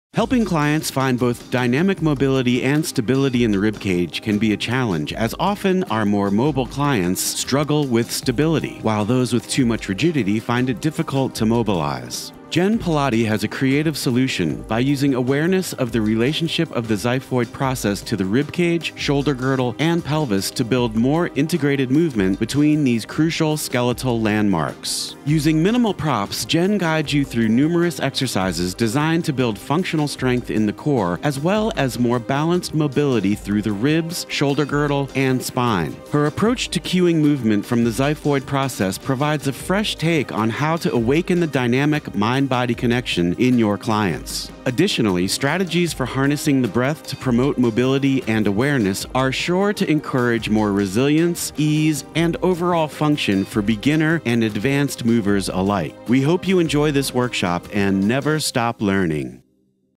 Xiphoid What? by Jenn Pilotti
This online Pilates workshop by Jenn Pilotti examines the relationship between the xiphoid process, the shoulder girdle, and the pelvis and how movement can be cued from this central part of the torso to create more awareness and integration.

Jen explores the anatomy and structures that play a role in the functional movement of the spine in all planes from the context of the xiphoid process. Mobility in the ribcage affects both the shoulder girdle and pelvis and spine. Balancing forces in these structures is crucial for activities of daily living as well as more advanced movements or athletic pursuits. As such, the exercises focus on mobility and stability of the torso and integrating/focusing the breath to encourage awareness and resilience.

Using minimal props, many exercises can be taught in the studio and assigned for a home program. This course is sure to provide you with specific, adaptable exercises that will bring integrated, functional awareness to clients of all ages and abilities.